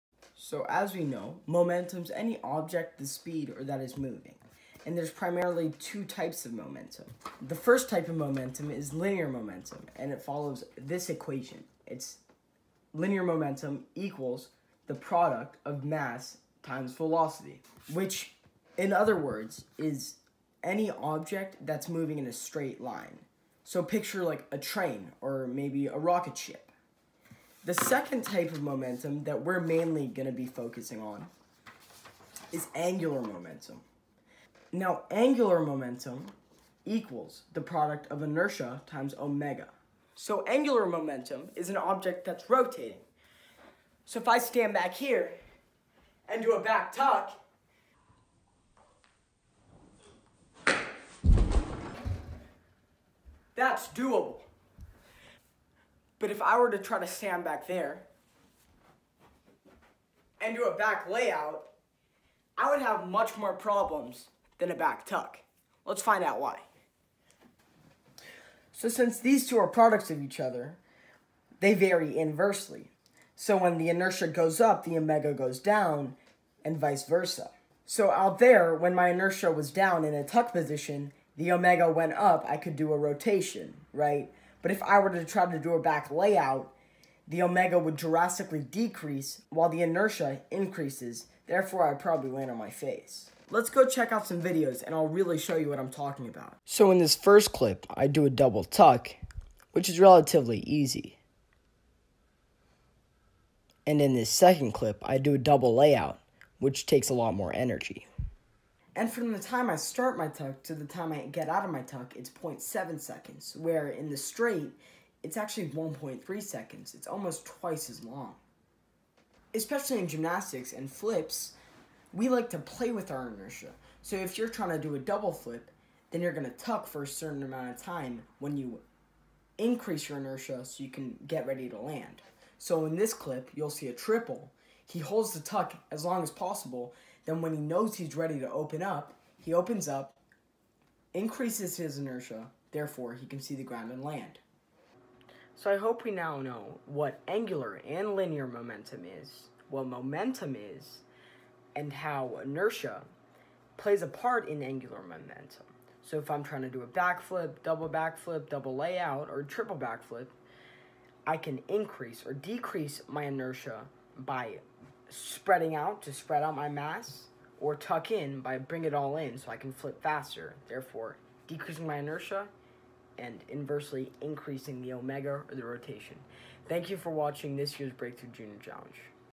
Breakthrough Junior Challenge 2019 | Angular Momentum
For this year's Breakthrough Junior challenge I decided to talk about momentum and how it affects a gymnast.